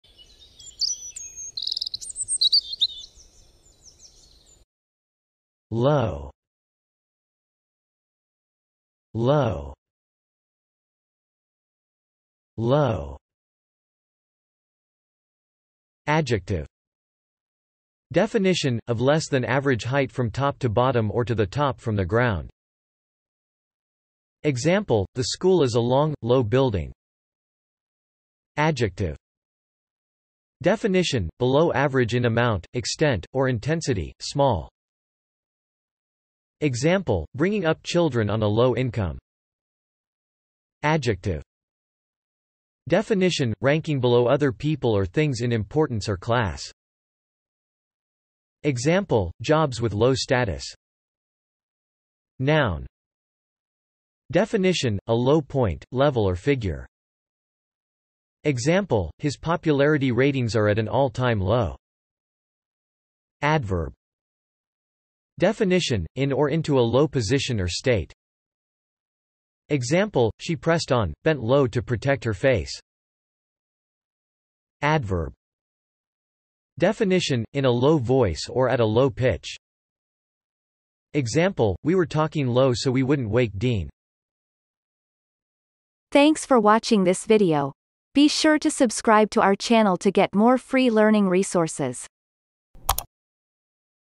How to pronounce low - Vocab Today
The pronunciation of the word low in Amercian accent is demonstrated in this video. If the word is from another language, such as brand name, it will be pronounced in its original language. Based on the word frequency, this is of 1 stars difficulty.

The word low has the following meanings:
1. (adjective)
Definition : Of less than average height from top to bottom or to the top from the ground.
Example : the school is a long, low building
2. (adjective)
Definition : Below average in amount, extent, or intensity; small.
Example : bringing up children on a low income
3. (adjective)
Definition : Ranking below other people or things in importance or class.
Example : jobs with low status
4. (noun)
Definition : A low point, level or figure.
Example : his popularity ratings are at an all-time low
5. (adverb)
Definition : In or into a low position or state.
Example : she pressed on, bent low to protect her face
6. (adverb)
Definition : In a low voice or at a low pitch.
Example : we were talking low so we wouldn't wake Dean